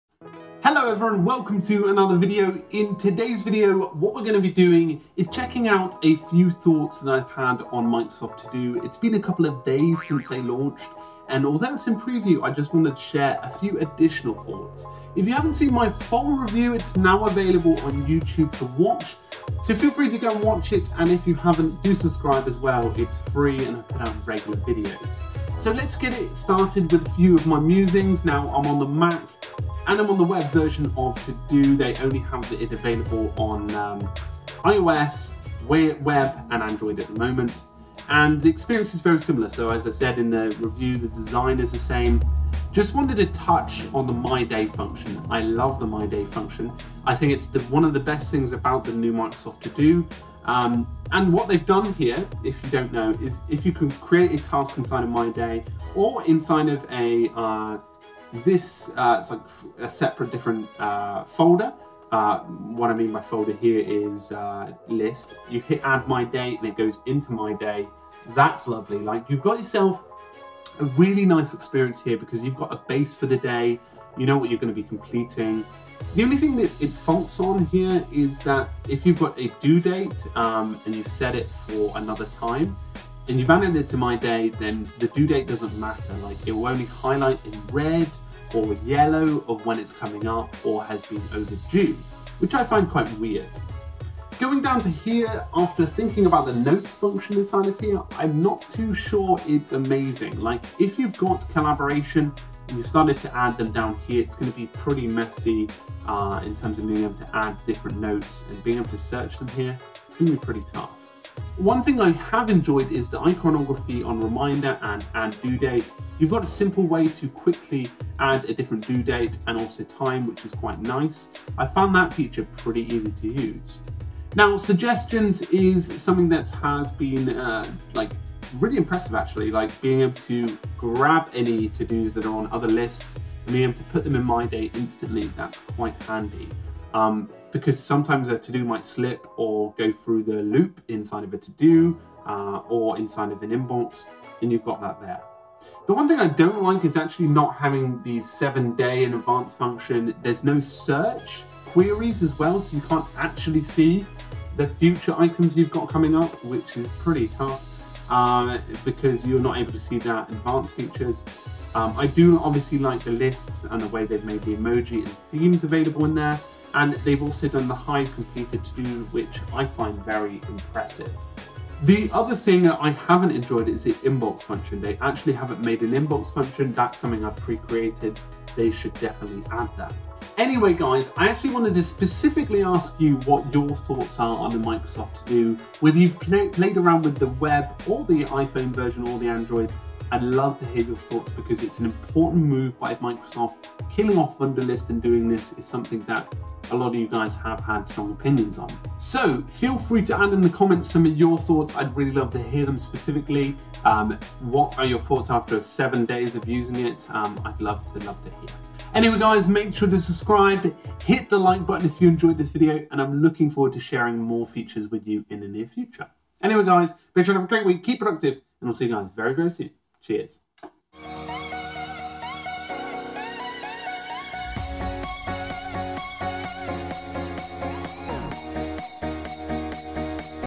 The Problems with Microsoft ToDo | No inbox!?
Microsoft ToDo has been out for 7 days now!

In today's video, we'll explore some of the problems inside of Microsoft ToDo and share the reasons why it's not up to scratch yet against Wunderlist!

Microsoft ToDo is available free here: todo.microsoft.com

// MUSIC

// ABOUT ME